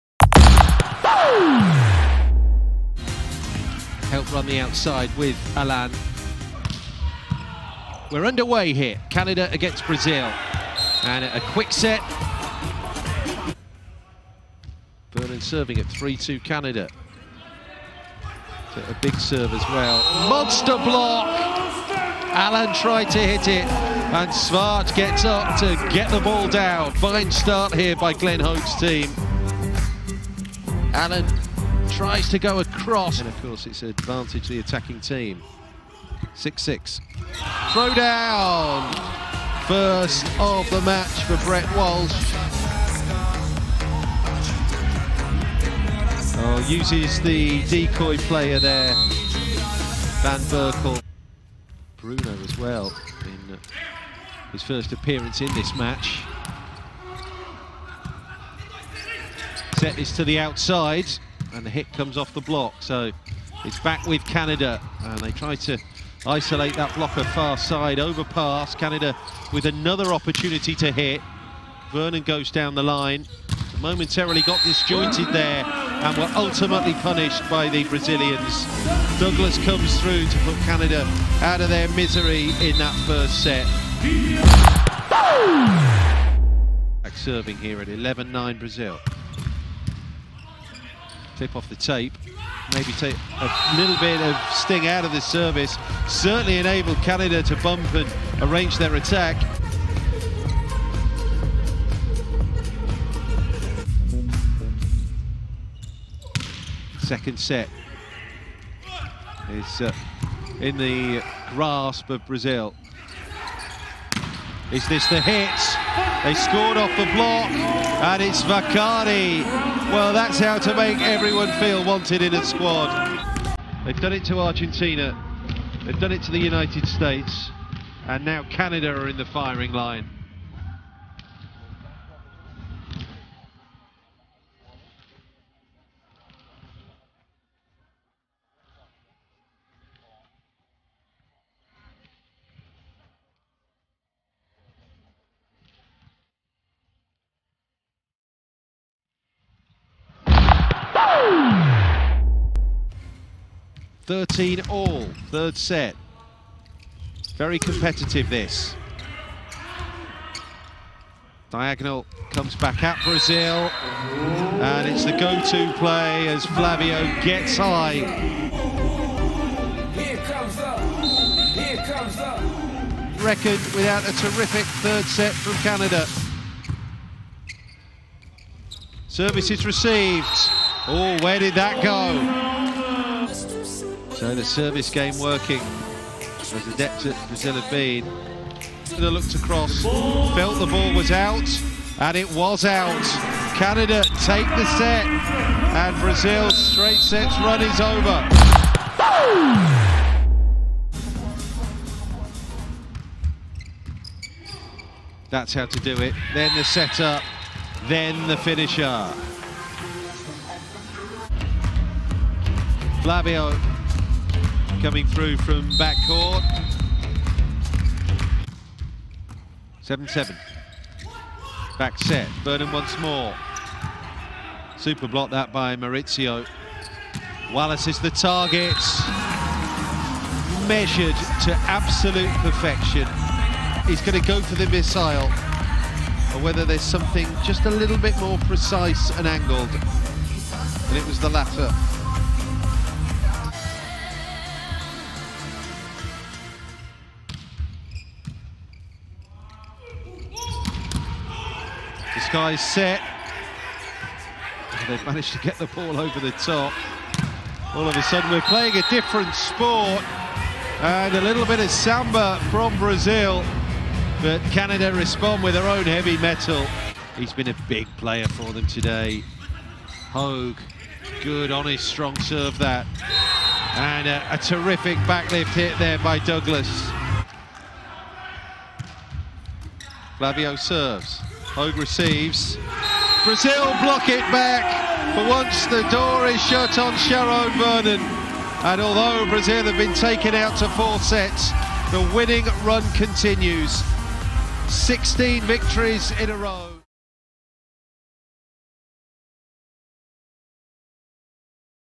CAN vs. BRA - Highlights Week 1 | Men's VNL 2021
Enjoy the Highlights from the men's match between CAN and BRA from Week 1 of the Men's VNL 2021 in Rimini (ITA).